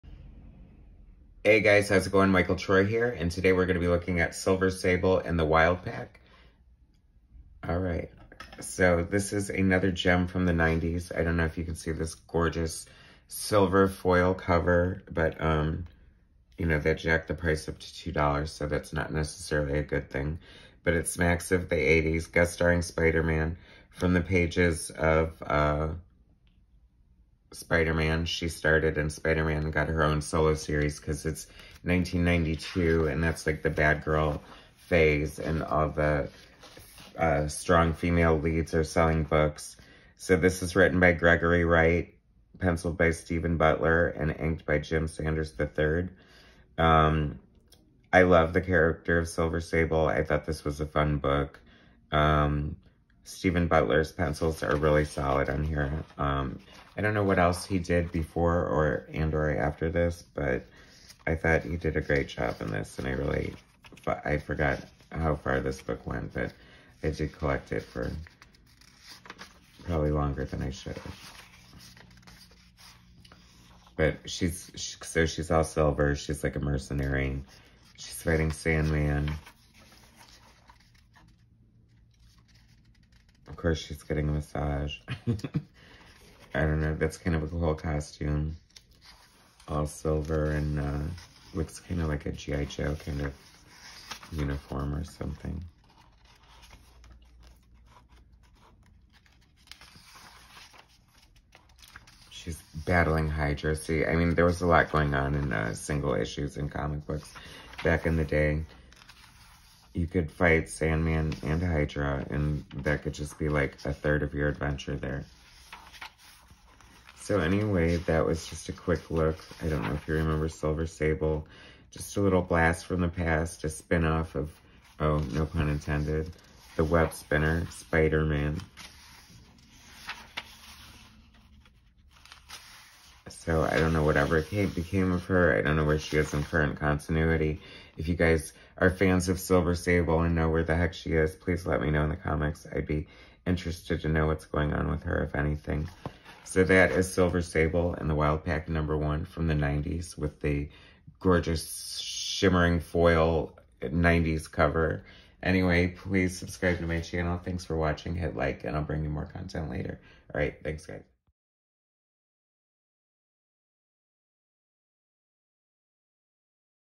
From the pages of Spider-Man! Silver Sable & The Wild Pack!
Another gem from the 90s. Silver Sable made her debut in Spider-Man comics. In the 90s, “Bad Girl” comics were all the rage so many female protagonists got their own titles. Silver Sable was a fun book that impressively lasted more than 30 issues. Join me for a look inside.